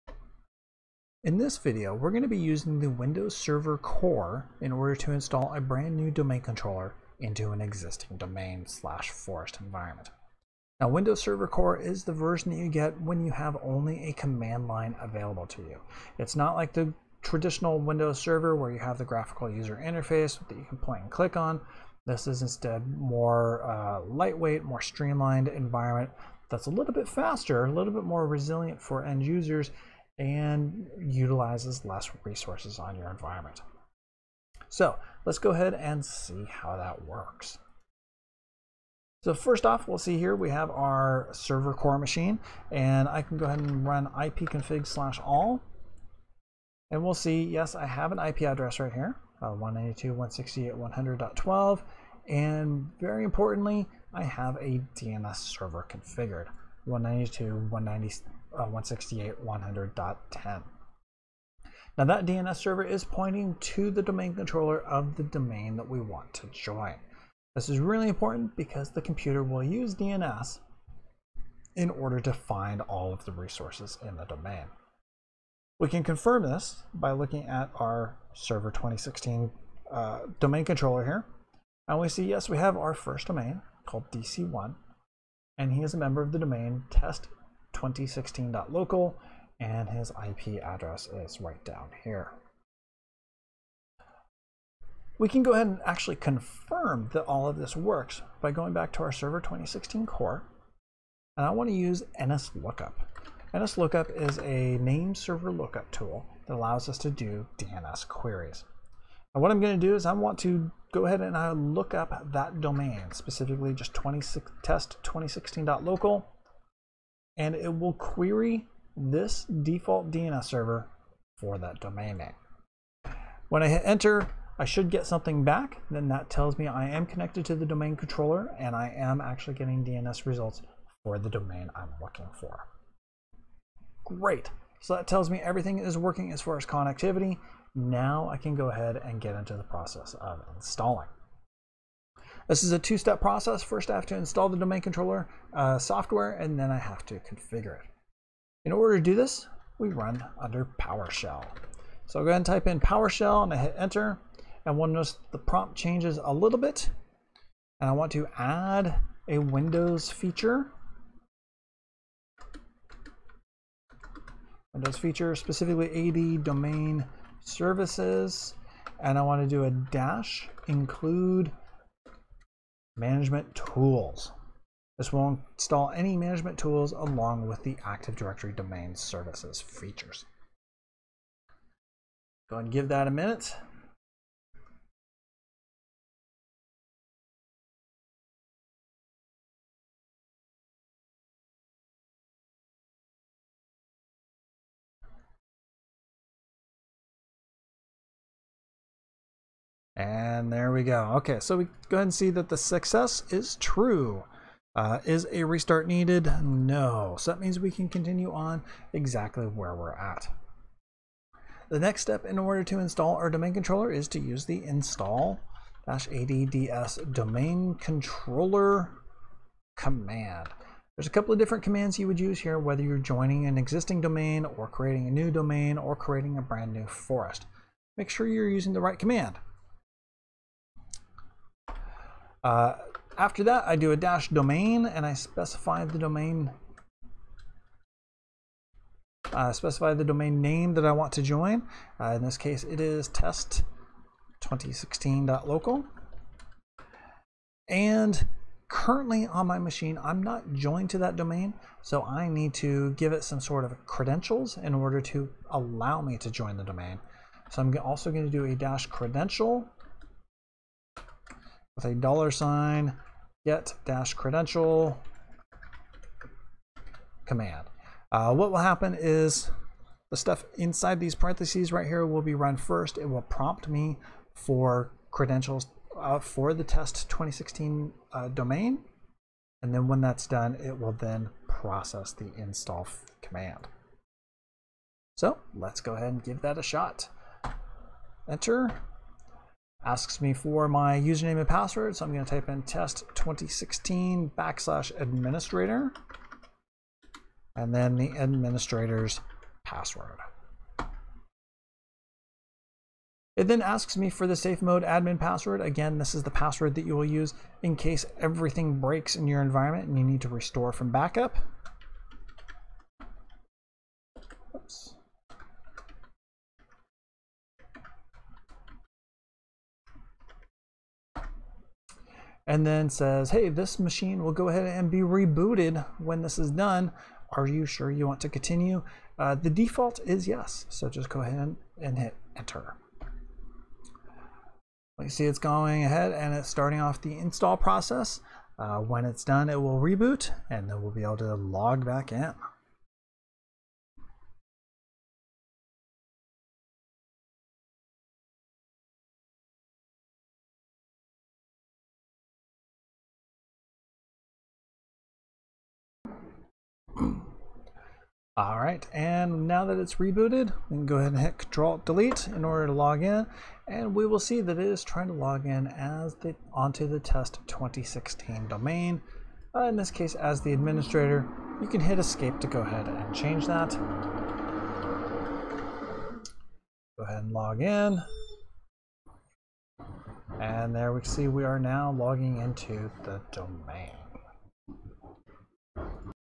Installing a new Server Core Domain Controller into an existing Domain/Forest
A walk-through of how to install a new Domain Controller on Windows Server Core. This example is joining the DC to an existing Domain/Forest environment.